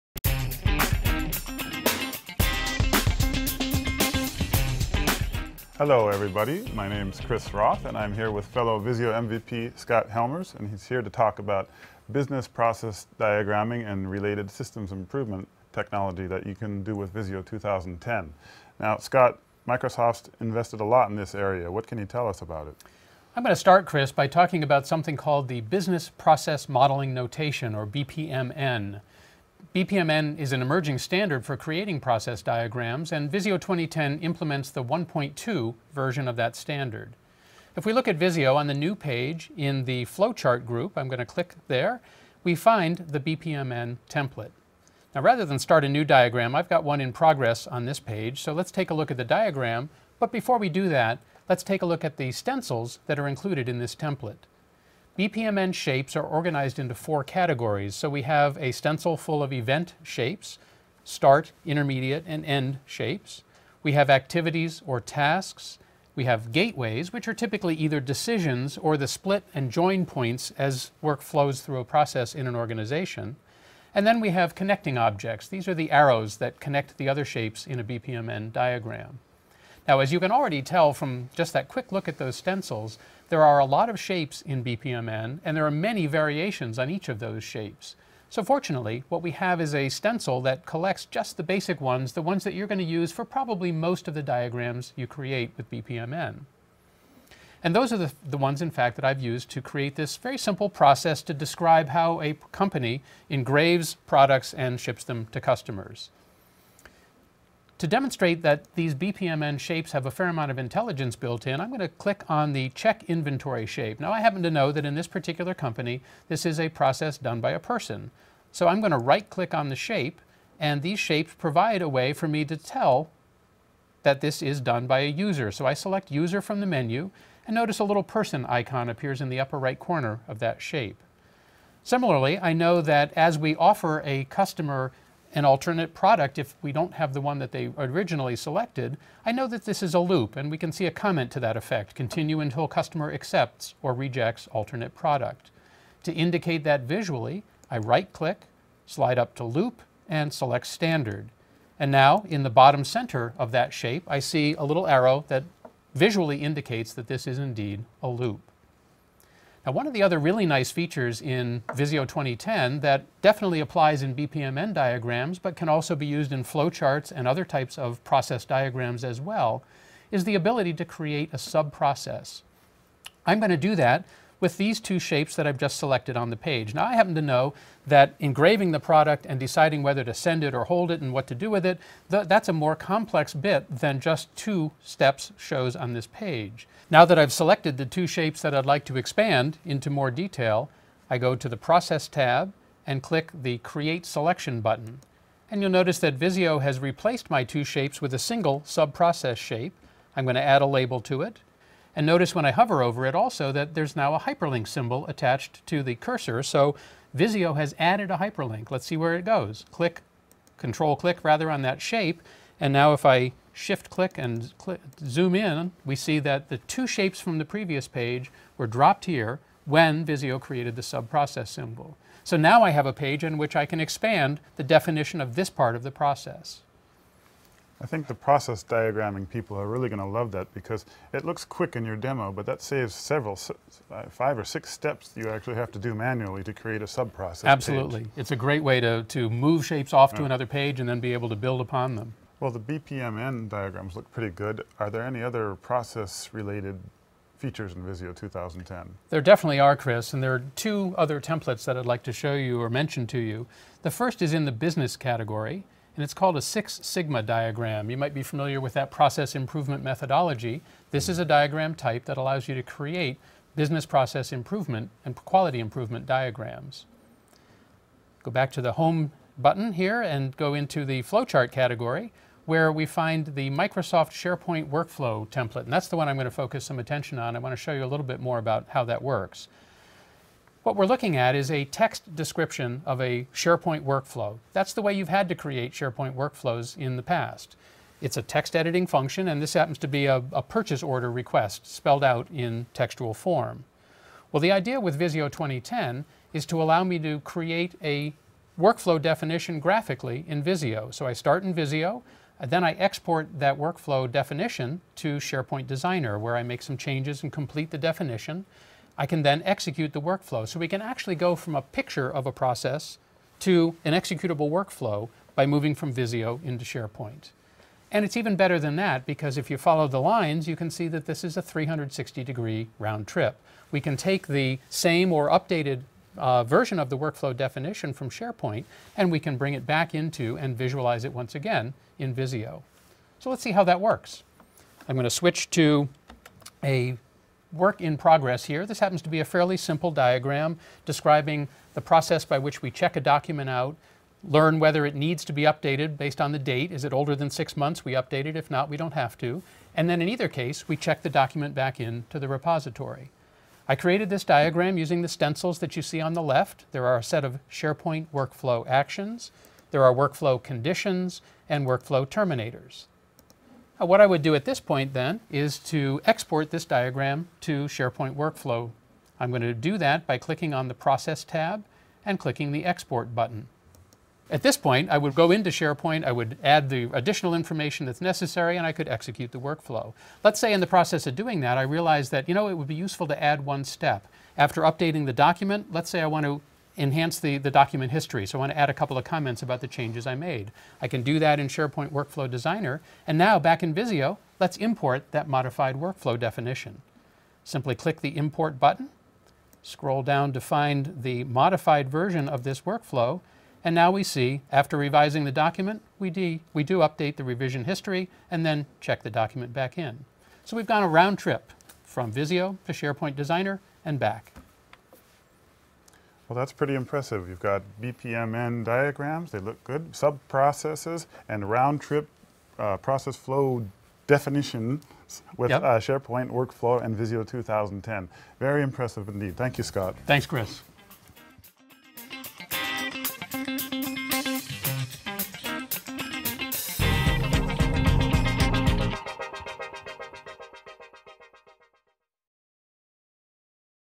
Session 20 Model BPMN and SharePoint workflows
Learn how to work with Business Process Modeling Notation (BPMN) diagrams and Microsoft SharePoint workflows in Microsoft Visio 2010. This video reviews the basics of working with BPMN diagrams, including features such as how to automatically create new pages for subprocesses. It also covers creating and modifying SharePoint workflows and demonstrates how they can be exported to and executed in SharePoint as well as modified in SharePoint and imported back into Visio.